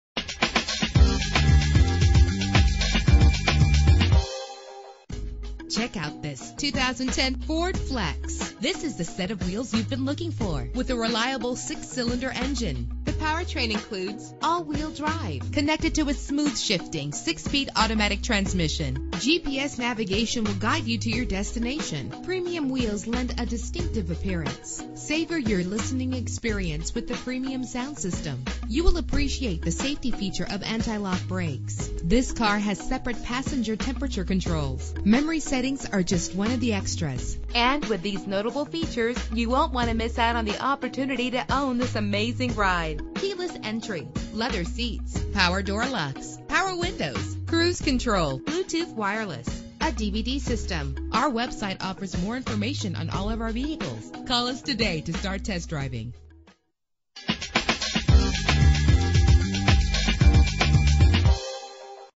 2010 Ford Flex - VERSAILLES KY
Year: 2010
Make: Ford
Model: Flex
Trim: Limited
Engine: 3.5 liter 6 cylinder 24 valve
Transmission: 6-Speed Automatic
Color: Maroon
Mileage: 28415
Address:
3405 LEXINGTON RD